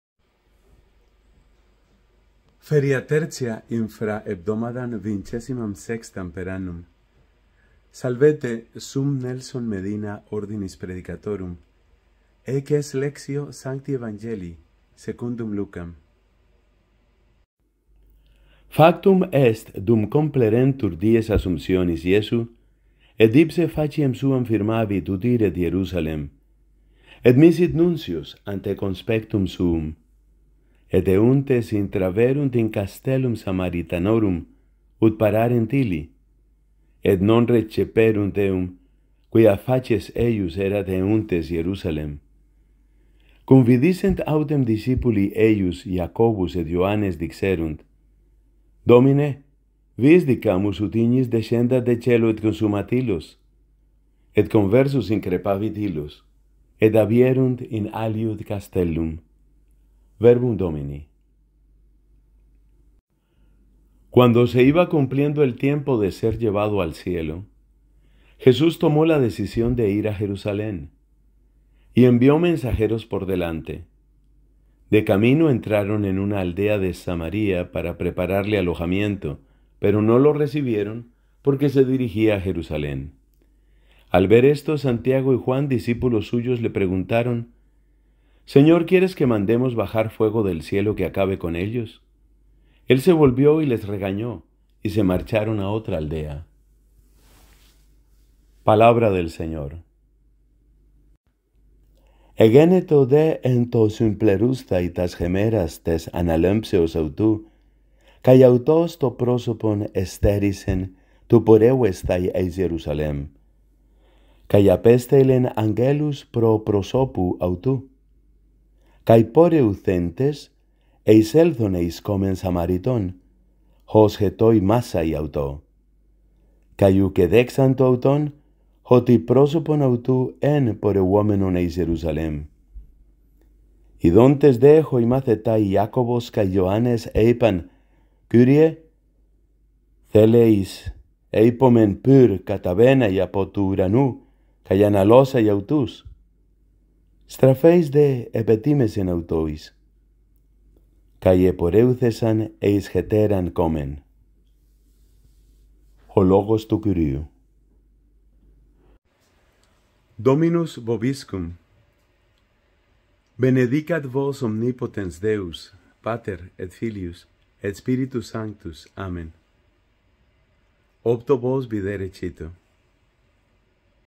i262 - Lucas 9, 51-56 - EVANGELIO DEL DÍA en Latín, Español y Griego
Martes XXVI del Tiempo Ordinario
00:17 Latín
01:03 Español
01:41 Griego